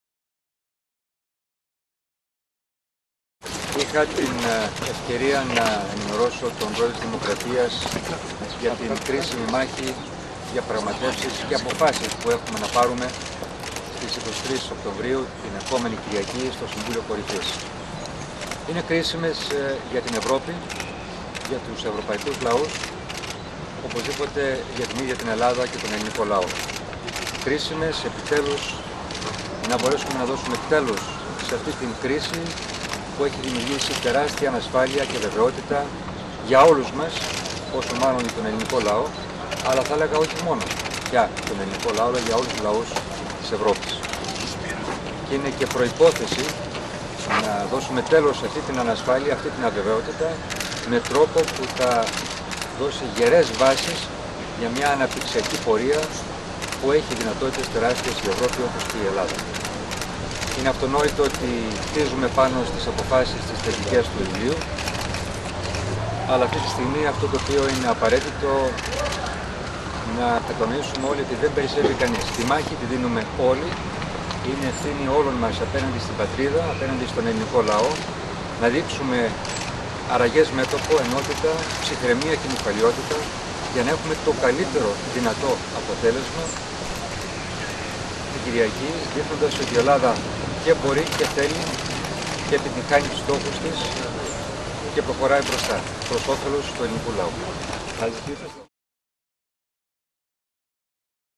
Δήλωση μετά τη συνάντηση με τον Κ. Παπούλια | 17.10.2011
Η  δήλωση του Πρωθυπουργού, Γιώργου Α. Παπανδρέου, μετά την συνάντηση του με τον Πρόεδρο της Δημοκρατίας Κάρολο Παπούλια.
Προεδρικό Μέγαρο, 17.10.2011